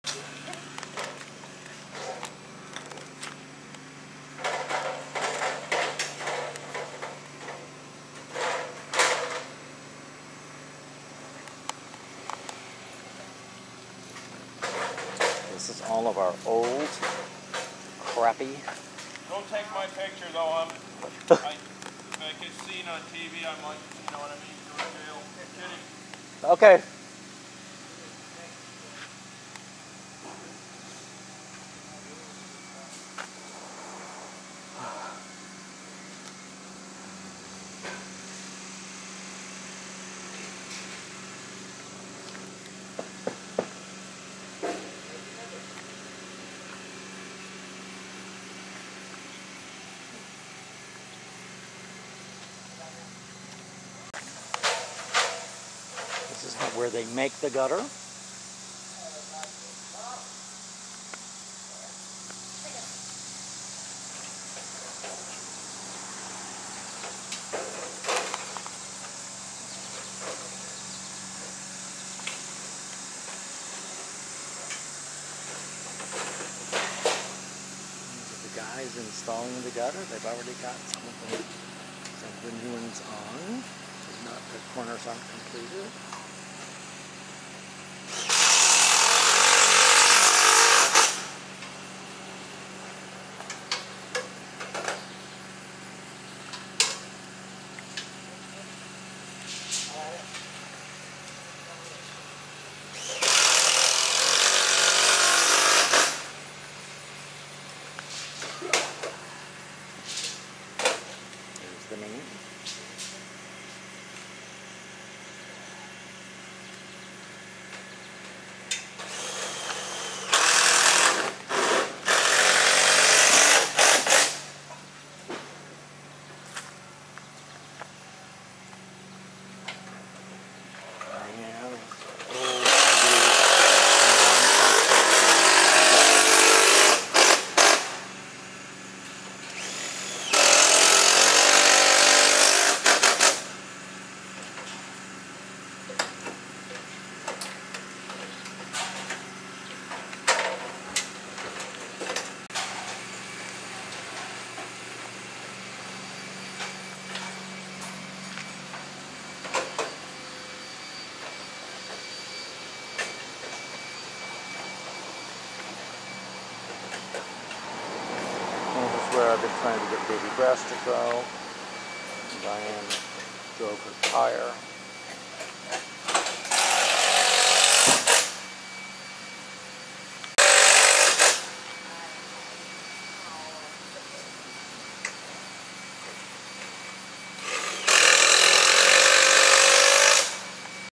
putting new gutters on our Lenexa house 7/21/10
This shows some parts of the process of our Lenexa, KS home in Colony Woods subdivision getting all new gutters on 7/21/10.  The reason for the new gutters was that the house needed a new roof, having been built in the late 70's, and but by around 2010, most of the homes in the subdivision either had already gotten or were showing signs of needing new roofs. Something that goes along with a new roof is new guttering because it simply takes too long to try and save the gutters when you are quickly taking off a roof. As you can see from the video, whoever designed this house, went for style as opposed to easy replacement of gutters. There are way more corners, short lengths, and other hand-made vs. off the shelf runs than one could possibly expect from a house of that size. Also, the pitch of the roof is steeper than most other house in the neighborhood as well. Our roofers, All Star Roofing, all in all did a super job and the extra cost was not nearly as much as I might have expected it to be. They did a really nice job of custom fitting all these tiny sections of gutter together and it ended up looking great. I didn't realize I had this footage until a few weeks ago and am posting it because we ended up needing a roof here in St. Louis too and the situation couldn't be more different--just 2 straight runs on only 2 sides of our St. Louis house as compared to dozens on the Lenexa house. I still can't comprehend how our roofers here were able to do so much custom work and get it all done in just one day--phenomenally efficient and fast on their part.